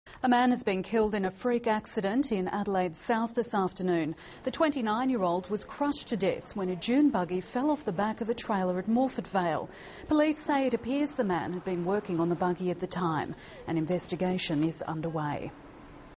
Man dies in freak accident
Police are investigating the death of a man who was crushed by a dune buggy in Adelaide's south.